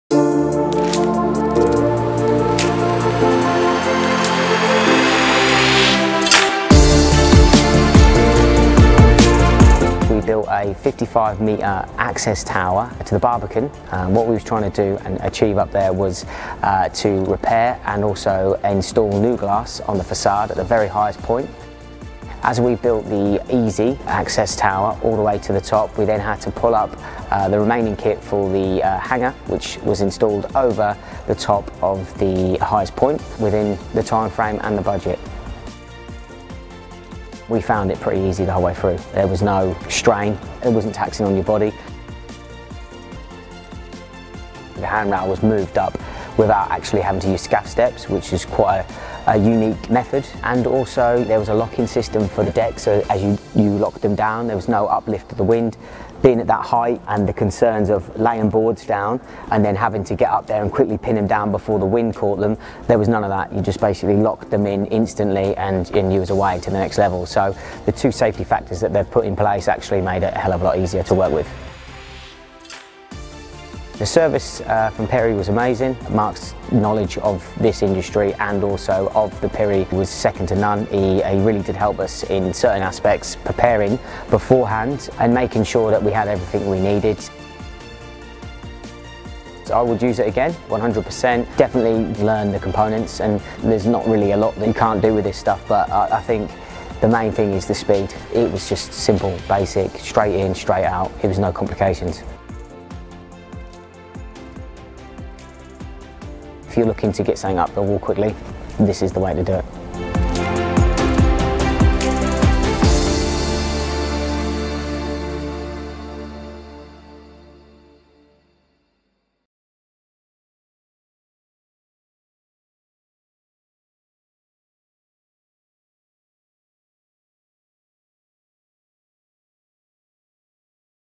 Blake Tower - Project Insight | PERI UK
PERI UP Scaffolding provides quick and easy access on Blake Tower, Barbican. Hear from our customer, Shaun Somerton - Director at Legion Scaffolding, who shares his experience of working with PERI UP systems on this project.